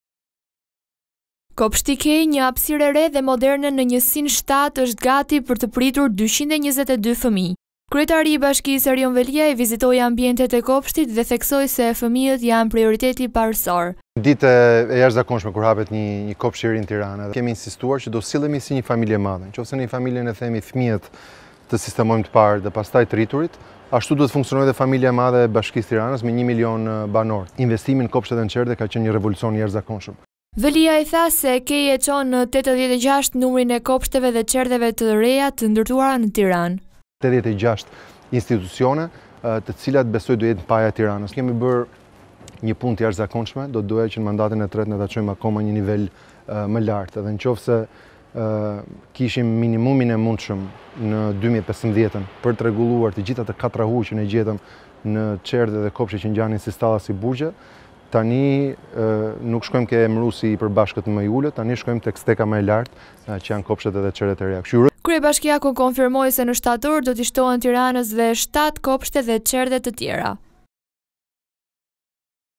Nje tjeter kopesht i shtohet Tiranes. Veliaj: femijet jane prioritet i Bashkise -
Kopshti “Kei” një hapësirë  e re dhe moderne në   njësinë 7  është gati për të pritur 222 fëmijë. Kryetari i bashkisë Erjon Veliaj  vizitoi ambientet e kopshtit dhe, theksoi  se fëmijët janë prioriteti parësor. Veliaj tha se “Kei” e çon në 86 numrin e kopshteve dhe çerdheve të reja të ndërtuara në Tiranë.Kryebashkiaku konfirmoi se në shtator do ti shtohen Tiranës edhe 7 kopshte dhe çerdhe  të tjera.